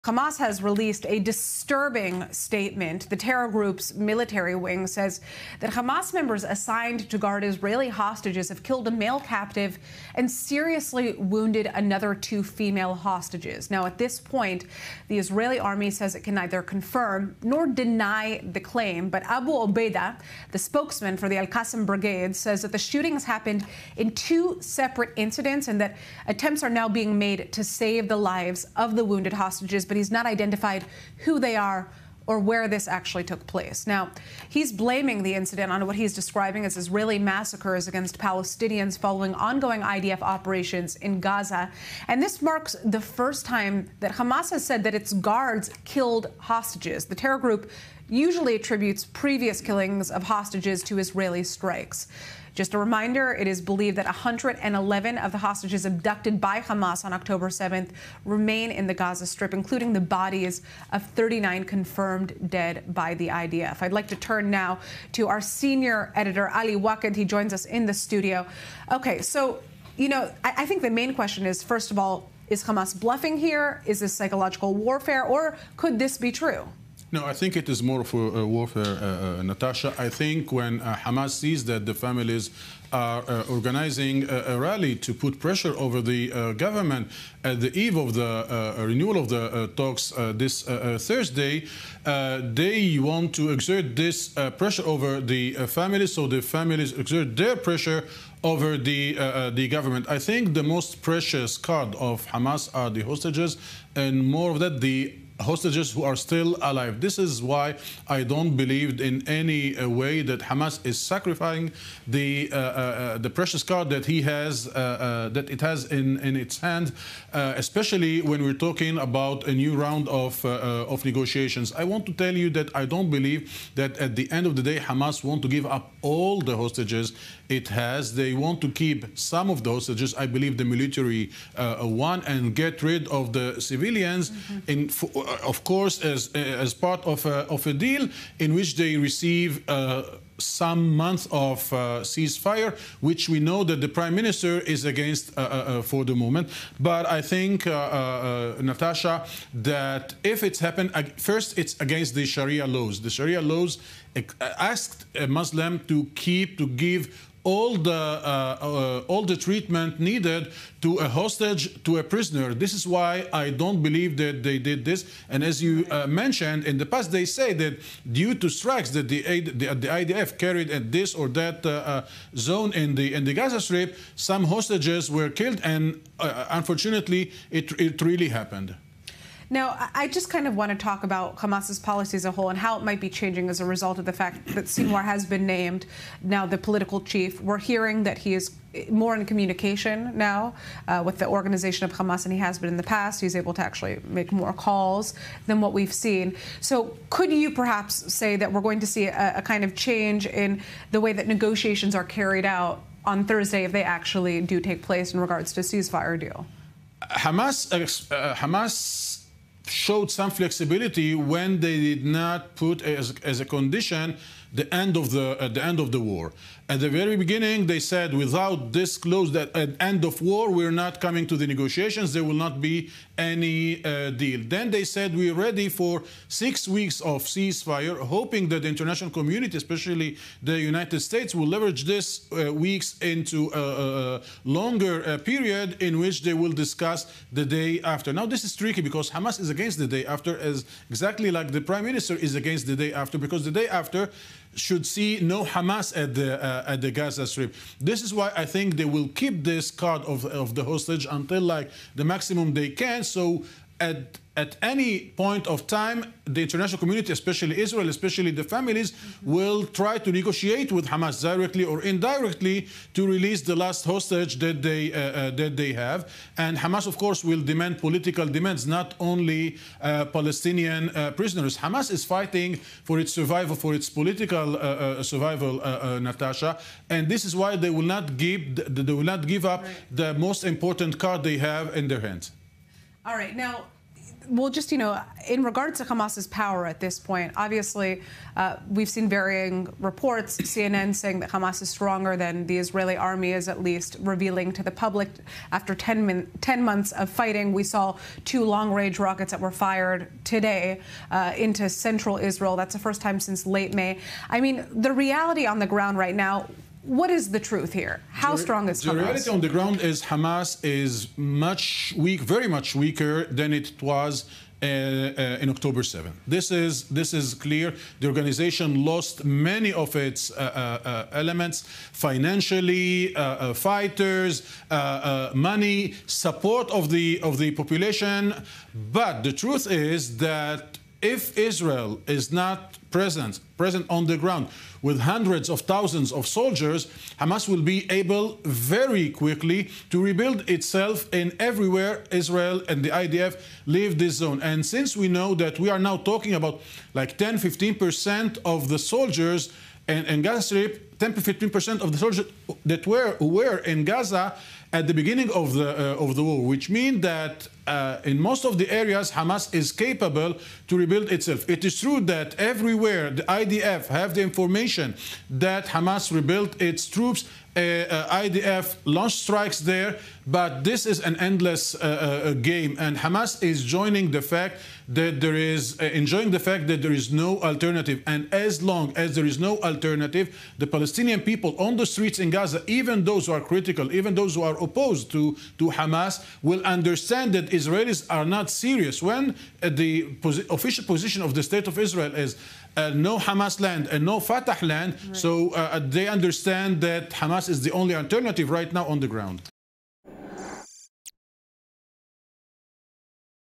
IDF opens probe after Hamas claims hostage killed
Hamas claims it has killed one male hostage and injured 2 female hostages, but is there any validity to this claim?